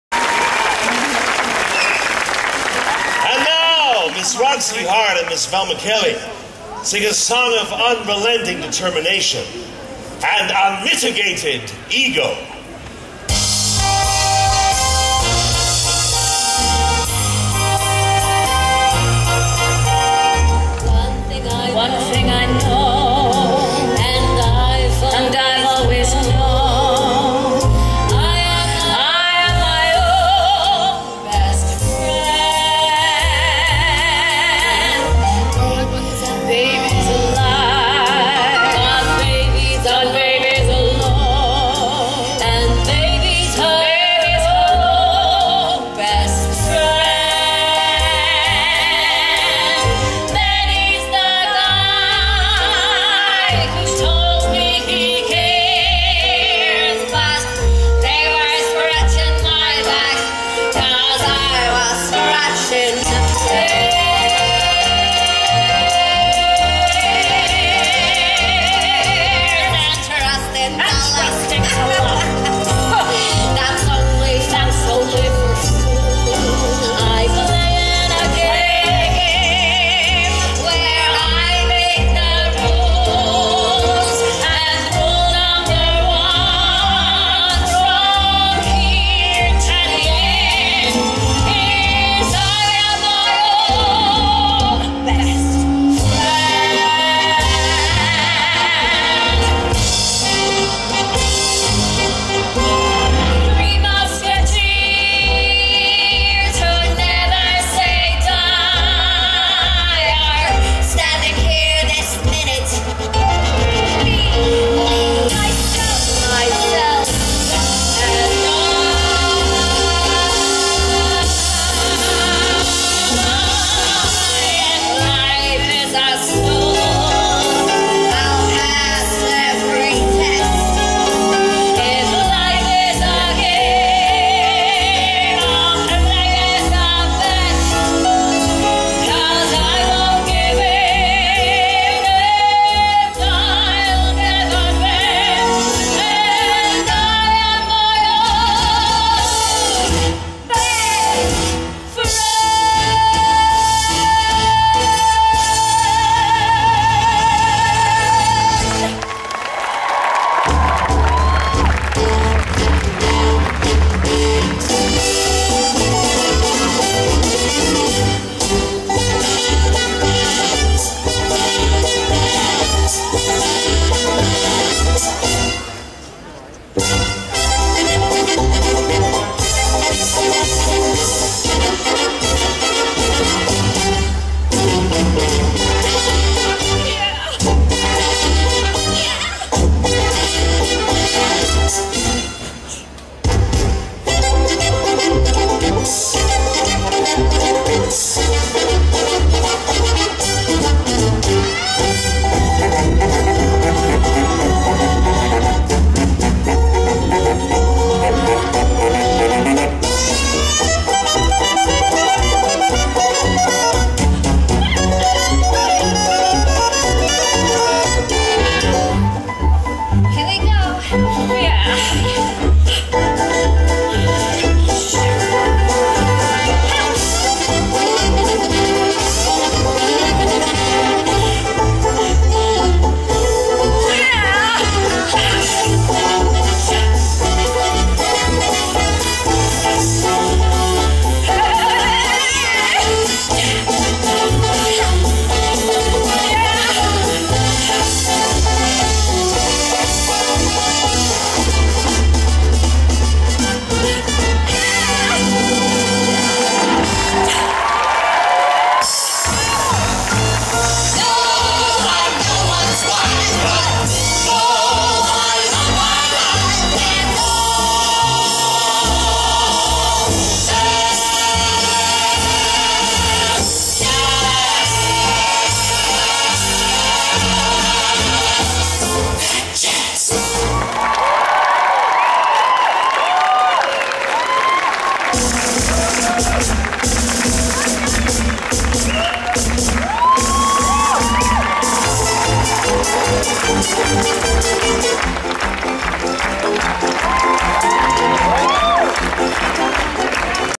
Anita Louise Combe and Sarah Soetaert as Velma & Roxie West End Live 2104
This video is about Anita Louise Combe and Sarah Soetaert as Velma & Roxie West End Live  2104